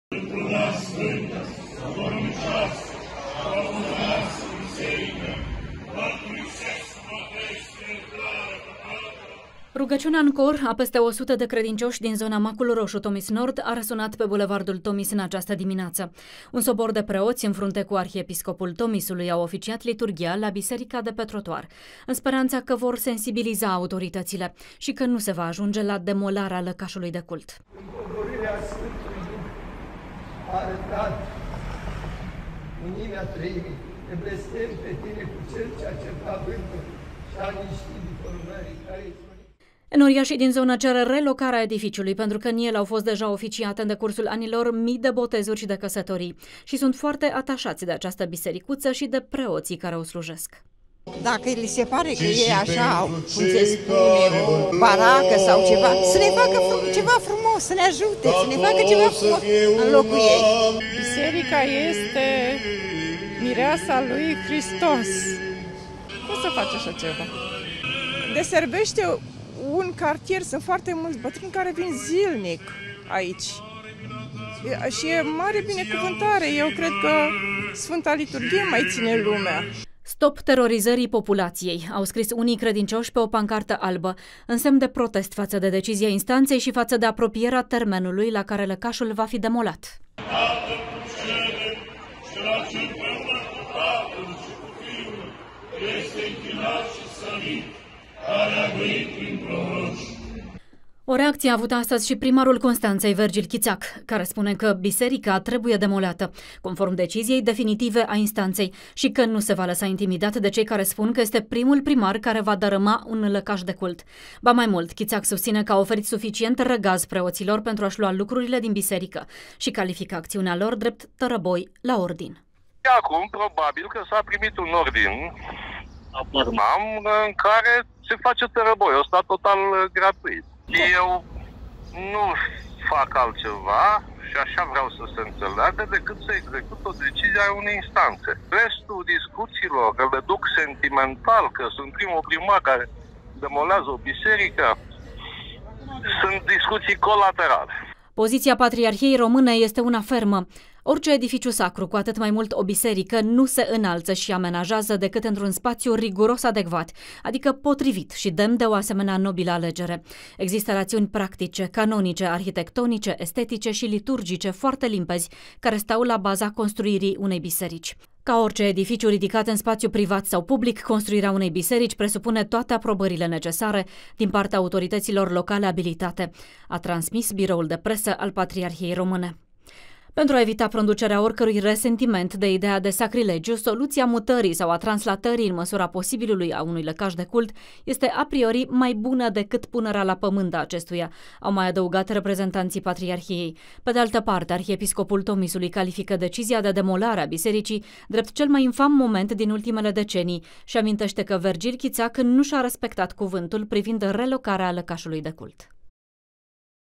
Slujbă împotriva demolării bisericii de pe trotuar
A fost slujbă împotriva demolării Bisericii de pe Trotuar, astăzi la Constanța.